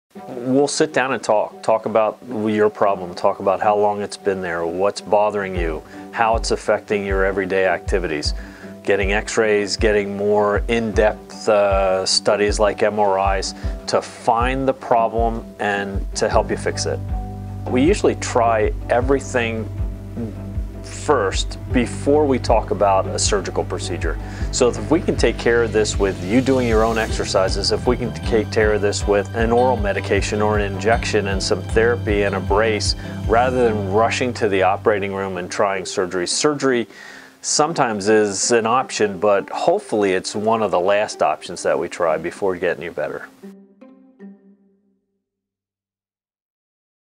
WHAT TO EXPECT ON YOUR FIRST VISIT - JEWETT ORTHOPAEDIC
At Jewett Orthopaedic Clinic, we look forward to meeting new patients and caring for their orthopaedic concerns. Here are a few tips that will help you know what to expect before your first appointment.